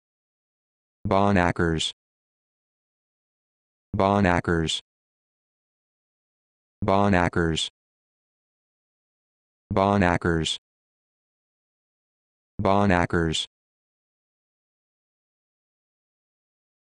How to Pronounce Bonackers
This video shows you how to pronounce Bonackers